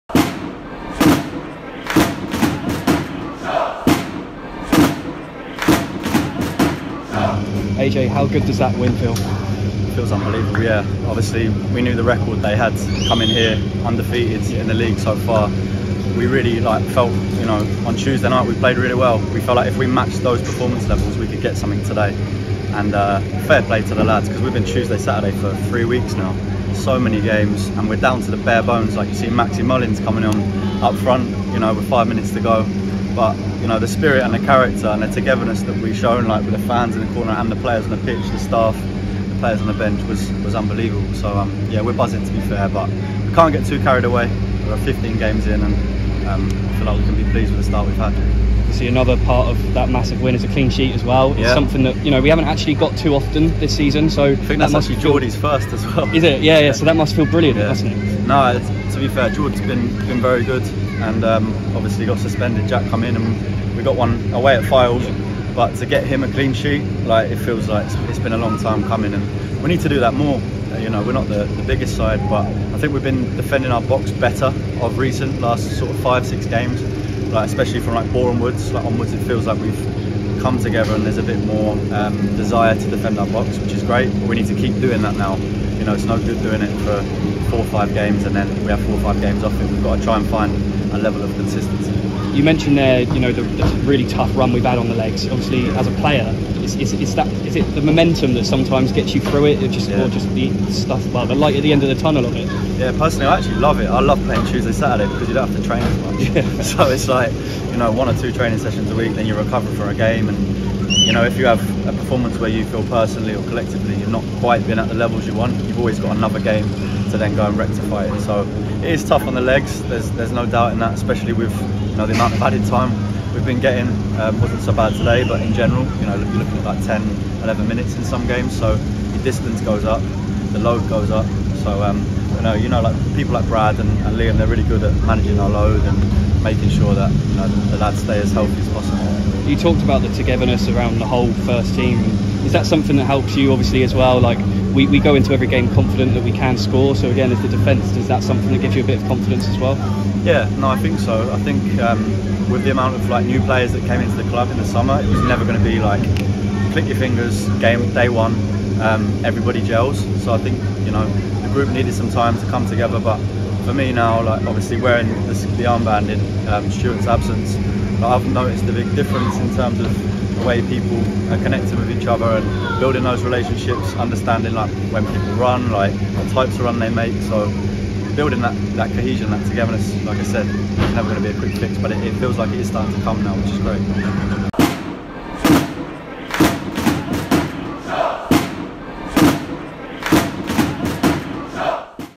AARON JONES POST-MATCH: Solihull Moors (A)
Aaron Jones speaks to ShotsTV following a 1-0 win over Solihull Moors!